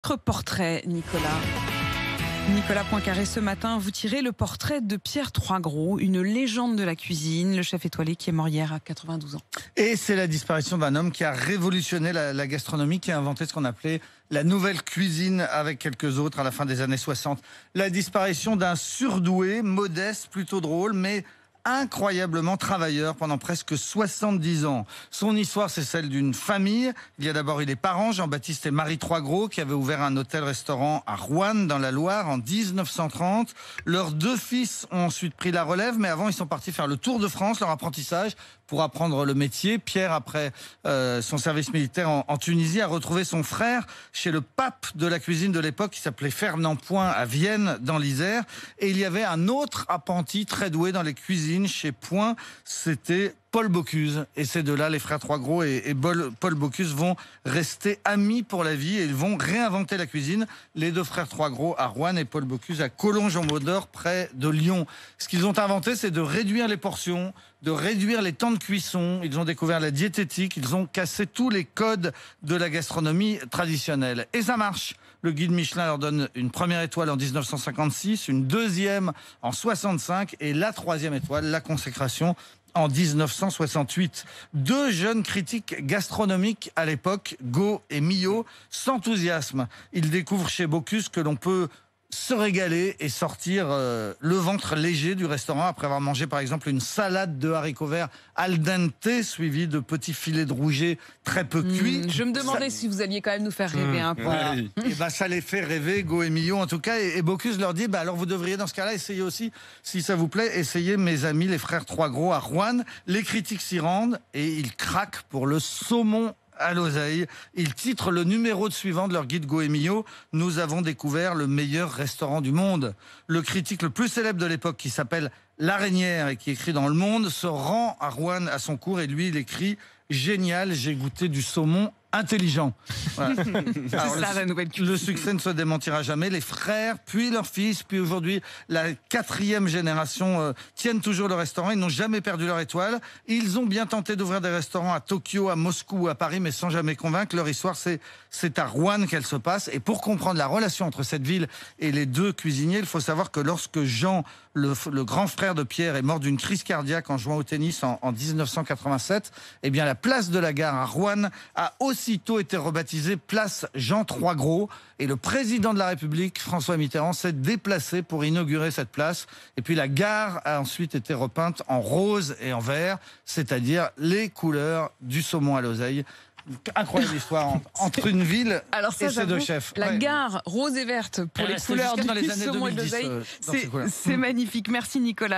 Le légendaire chef trois étoiles Pierre Troisgros est mort
LE PORTRAIT DE POINCA - Le monde de la gastronomie est en deuil: Pierre Troisgros, devenu célèbre avec son frère Jean pour avoir révolutionné la cuisine et fait de l'hôtel-restaurant familial de Roanne (Loire) un monument de la bonne chère à la française, est décédé à l'âge de 92 ans. Son portrait avec Nicolas Poincaré